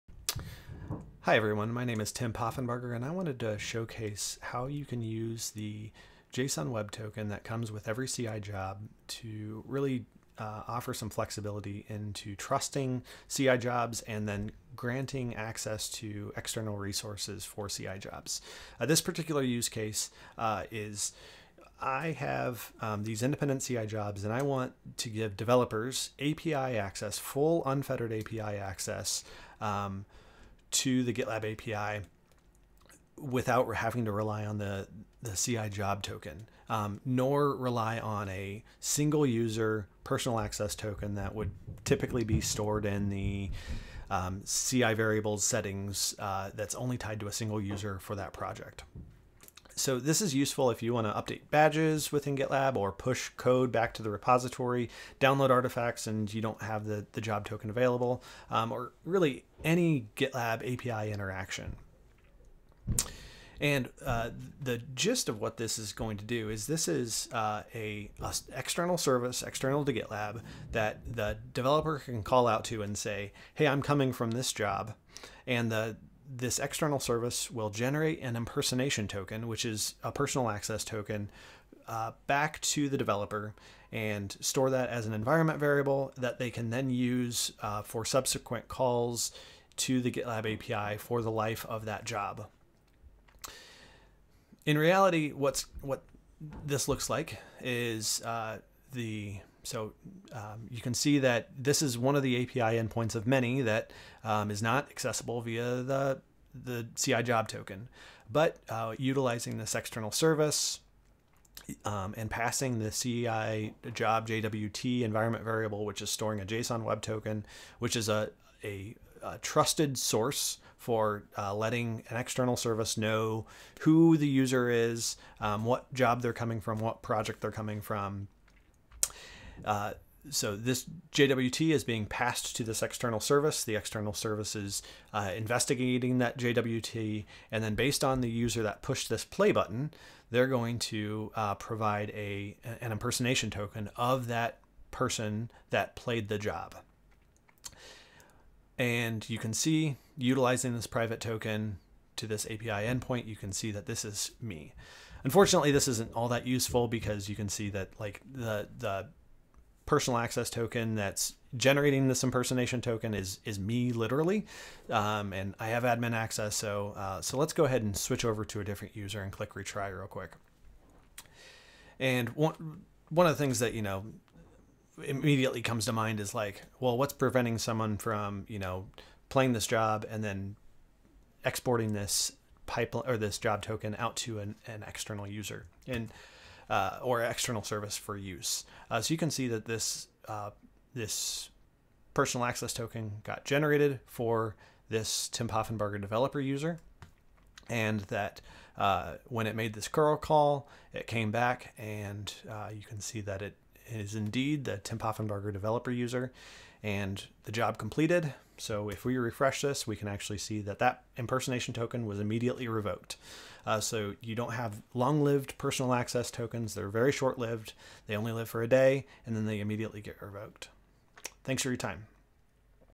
Personal Access Token Generator
Have you ever wanted more access to the GitLab API within a GitLab CI Job but not rely on a single person's Personal Access Token?

That's the aim of this preview. It also highlights the power of the CI Job's JSON Web Token and how it can be leveraged to grant CI Jobs access to external resources in a trustworthy way.